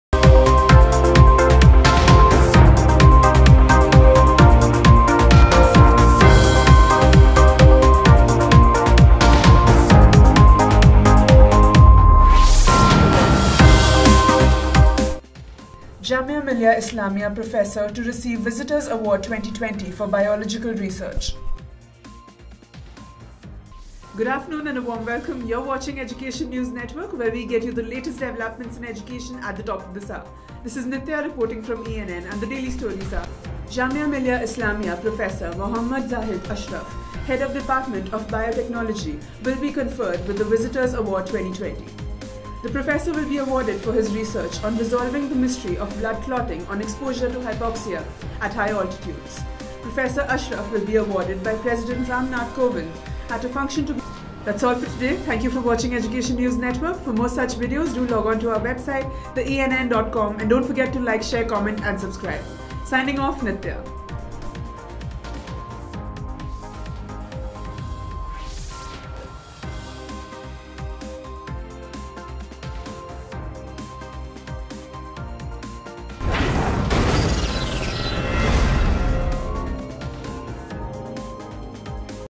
Jamia Millia Islamia Professor to receive "Visitor’s Award 2020" - Educational News
Jamia Millia Islamia (JMI) Professor Mohd Zahid Ashraf, Head of Department of Biotechnology will be conferred with the Visitor’s Award 2020. The professor will be awarded for his research on resolving the mystery of blood clotting on exposure to hypoxia at high altitudes. Professor Ashraf will be awarded by President Ram Nath Kovind at a function to be organised at the Rashtrapati Bhavan.
Professor Ashraf will receive this award under the Biological Sciences category. His work, as per a JMI statement, has contributed towards developing strategy for an early diagnosis and treatment of thrombosis at extremely challenging environmental conditions at high altitudes.